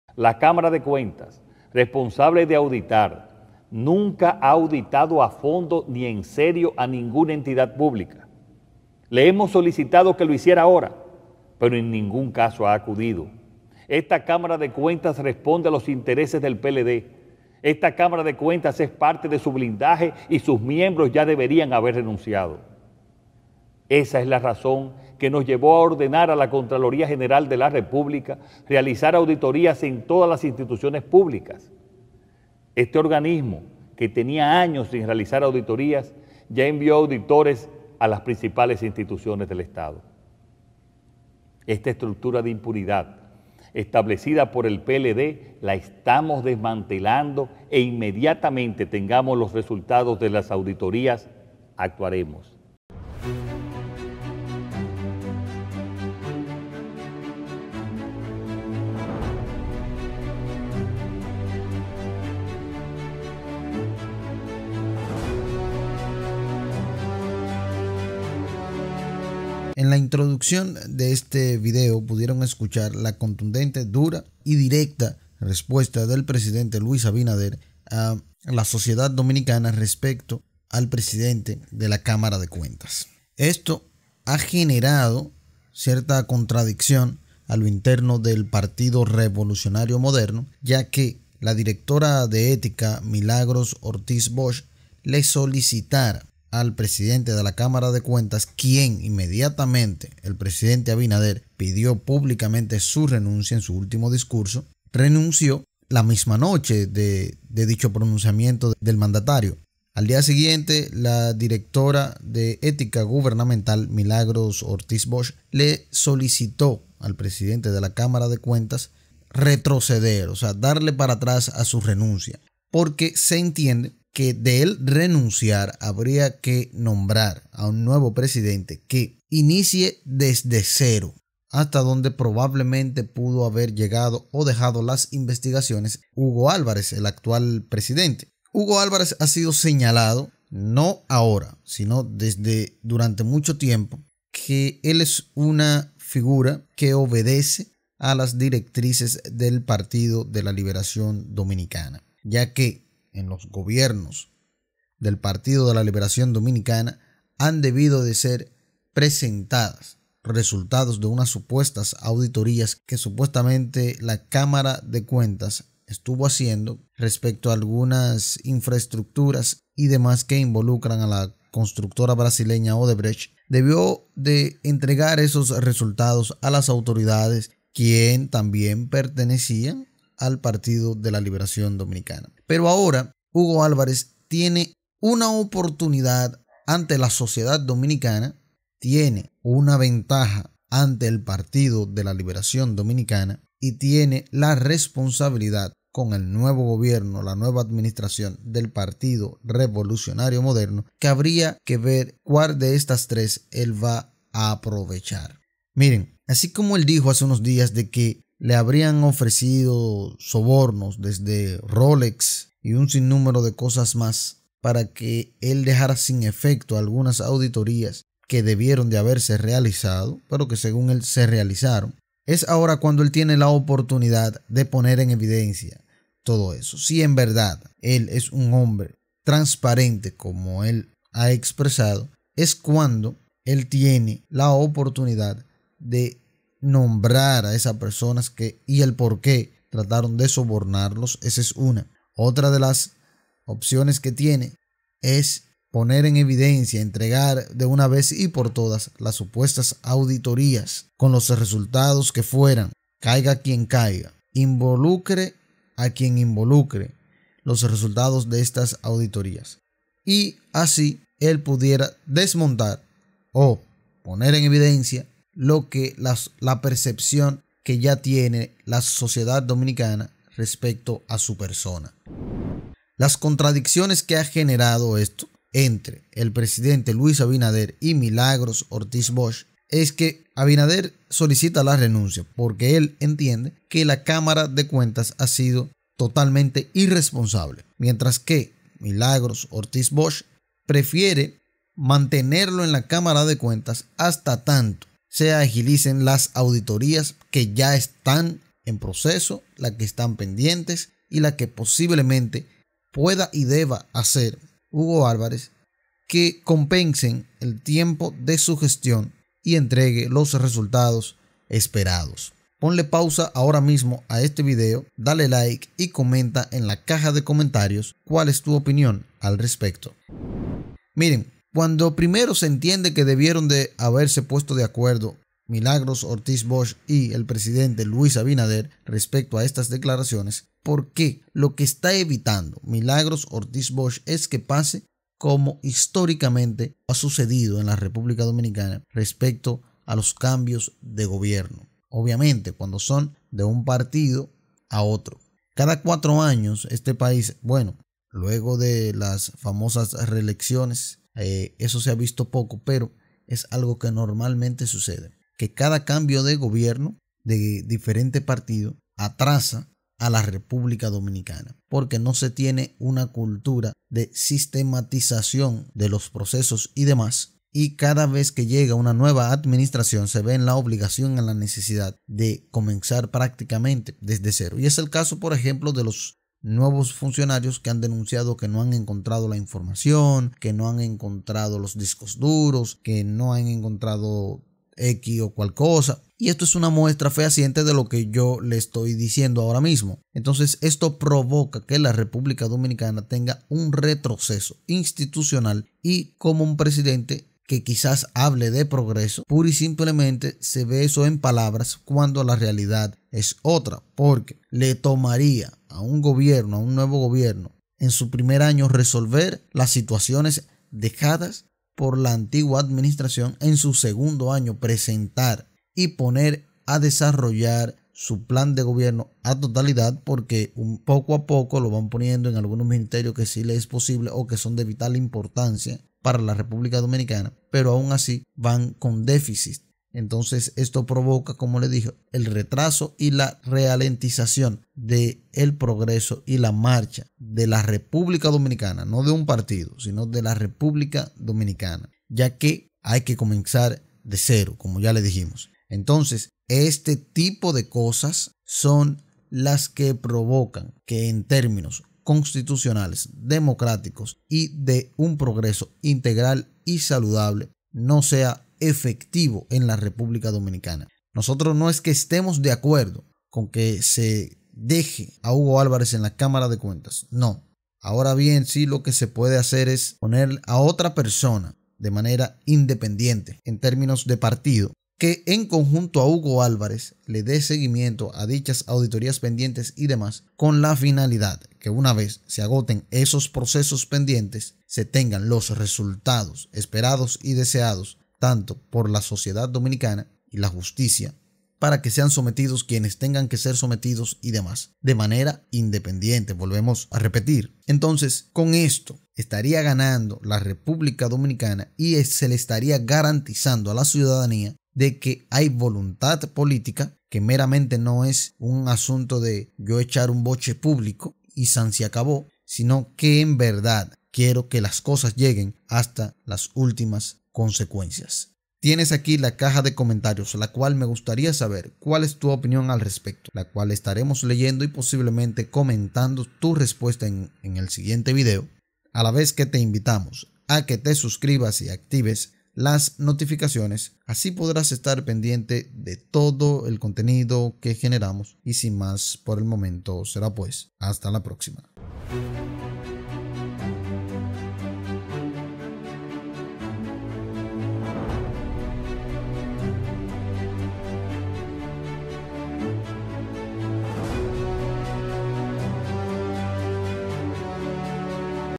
Hugo Álvarez (ES PRESIONADO) para que tire pa´lante al PLD…
En su discurso al país para ofrecer detalles sobre el anteproyecto de Presupuesto, los impuestos contemplados y otros temas, Abinader afirmó que la Cámara de Cuentas «nunca ha auditado a fondo ni en serio a ninguna entidad pública».

«Le hemos solicitado que lo hiciera ahora, perno en ningún caso ha acudido», enfatizó el jefe del Estado quien indicó que esa fue la razón que los llevó a ordenar a la Contraloría General de la República a realizar la auditoría en todas las instituciones públicas.

«Este organismo que tenía años sin realizar auditorías ya envió auditores a las principales instituciones.Esta estructura de impunidad establecida por el PLD la estamos desmantelando e inmediatamente tengamos los resultados de las auditorias actuaremos», agregó.

La directora de Ética Gubernamental, Milagros Ortiz Bosch, envió este lunes una carta al presidente de la Cámara de Cuentas, Hugo Álvarez Pérez, a quien le solicita que no renuncie, a pesar de las duras críticas públicas que le hizo el presidente Luis Abinader.